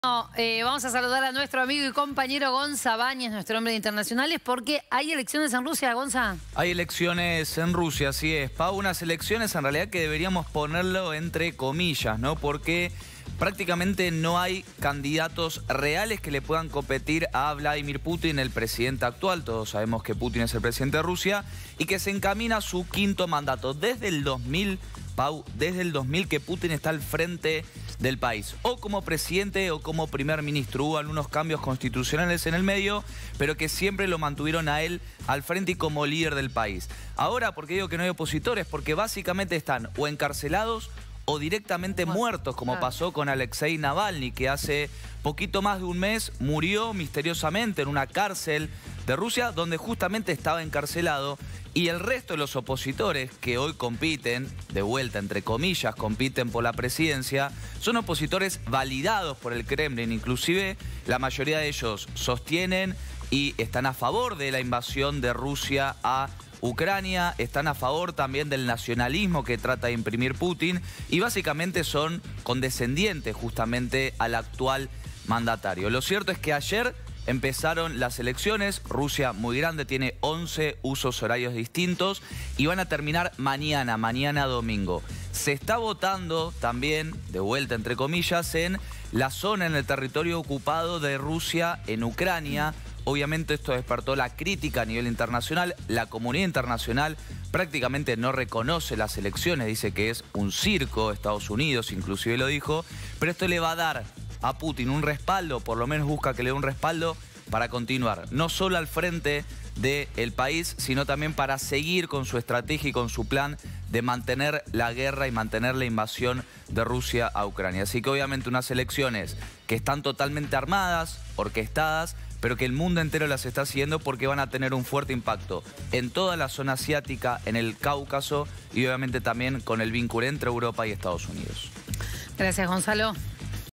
ELECCIONES EN RUSIA I Sin opositores, triunfo asegurado de Putin
Desde antes de que empezaran a abrir los centros de votación en distintas ciudades de Rusia esta mañana, Vladimir Putin ya sabía que seguirá al frente del país por seis años más. Desde este viernes los rusos votan en unas elecciones presidenciales de tres días, marcadas por la ofensiva en Ucrania y donde no hay opositores reales por persecución y encarcelamiento.

Putin, de 71 y años y en el poder desde el año 2000 como presidente o primer ministro, busca convertir los comicios en una demostración de apoyo a su ofensiva militar en Ucrania, que el mes pasado cumplió dos años.